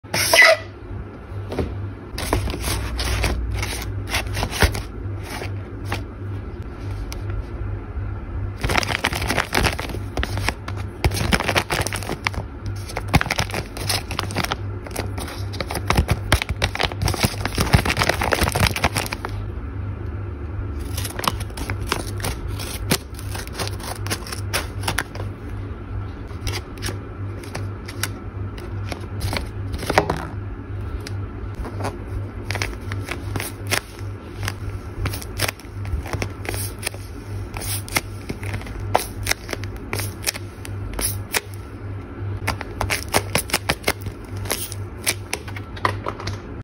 Relaxing ASMR Unboxing Dinosaur Blind Bag!🦖🦖
Hey guys!🤩🦖 Today I unboxing dinosaur blind bag!🦖✨ Enjoy the video!🤍🤍

Help me reach my goal!